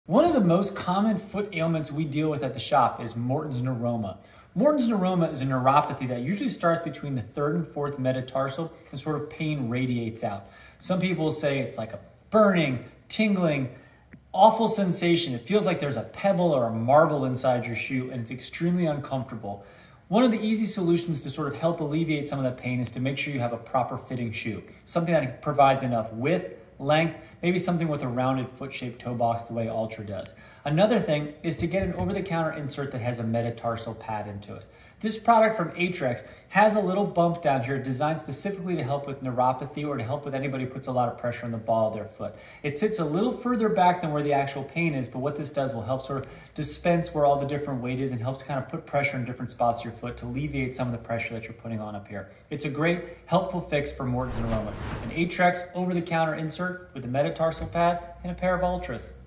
Shoe Inserts for Morton's Morton's Neuroma | What can help my Morton's Neuroma?
What are the best shoe inserts to help with Morton's Neuroma?  Help your Morton's Neuroma by buying the right shoe and right shoe insert.